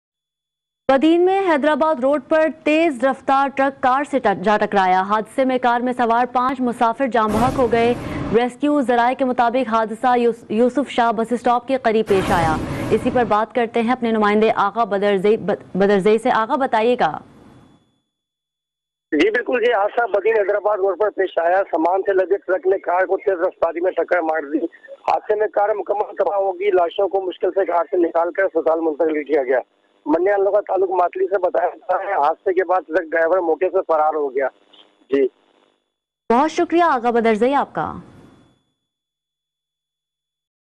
بدین: حیدرآباد روڈ پر تیز رفتار ٹرک کار سے جا ٹکرایا حادثے میں کار سوار پانچ افراد جاں بحق۔
Video from Agha